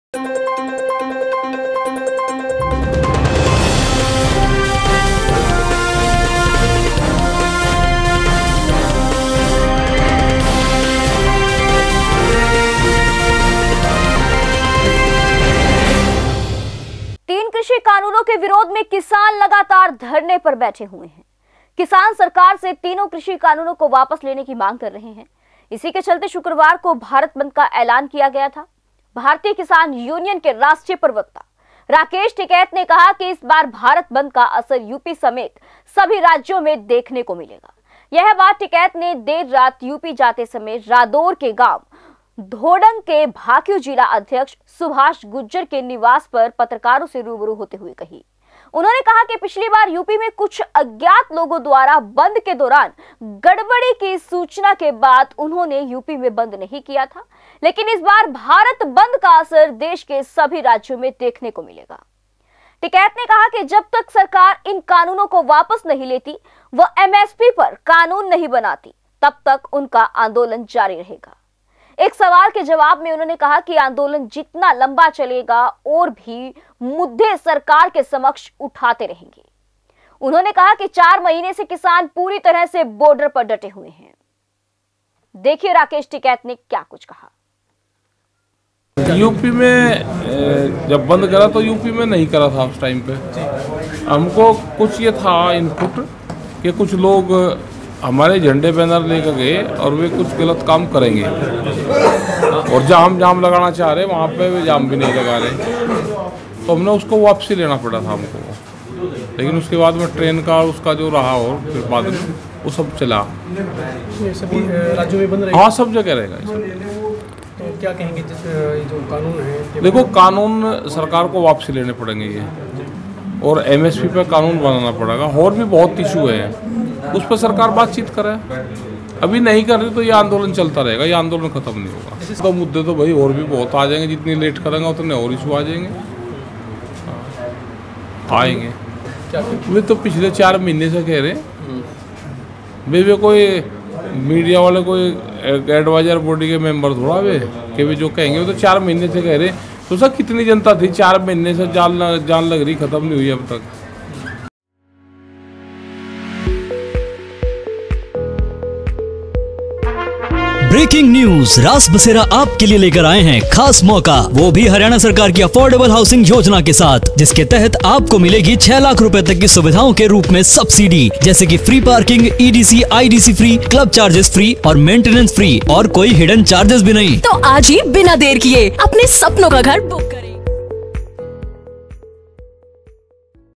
यूपी समेत देश के सभी राज्यों में दिखेगा भारत बंद का असर राकेश टिकैत
तीन कृषि कानूनों के विरोध में किसान लगातार धरने पर बैठे हुए हैं। किसान सरकार से तीनों कृषि कानूनों को वापस लेने की मांग कर रहे हैं। इसी के चलते शुक्रवार को भारत बंद का ऐलान किया गया था। भारतीय किसान यूनियन के राष्ट्रीय प्रवक्ता राकेश टिकैत ने कहा कि इस बार भारत बंद का असर यूपी समेत सभी राज्यों में देखने को मिलेगा। यह बात टिकैत ने देर रात यूपी जाते समय रादौर के गांव धौडंग में भाकियू जिलाध्यक्ष सुभाष गुर्जर के निवास पर पत्रकारों से रूबरू होते हुए कही